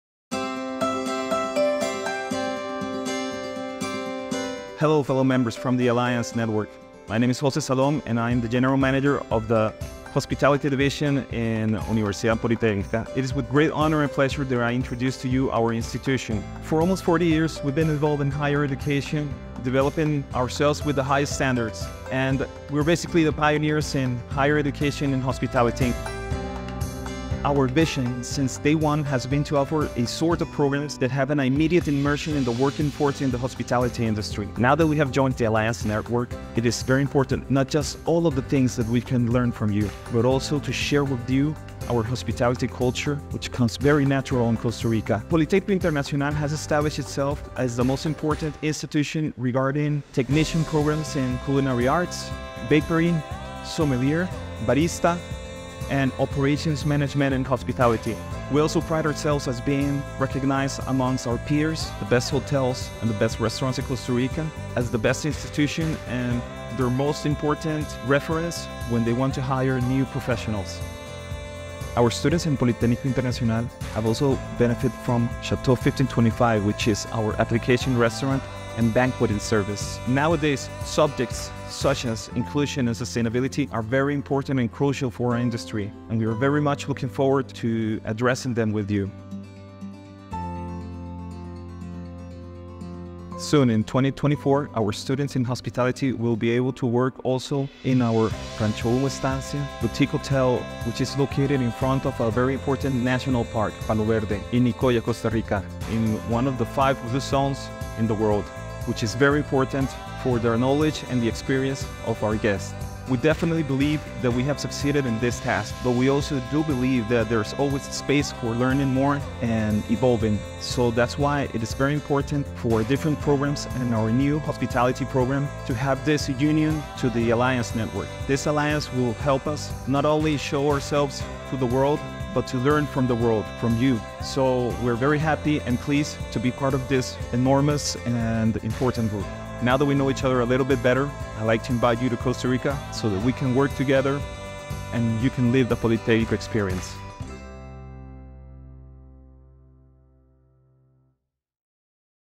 Instituto Politécnico Internacional - Universidad Politécnica Internacional X Alliance Network
Presentation of Instituto Politécnico Internacional by José Salom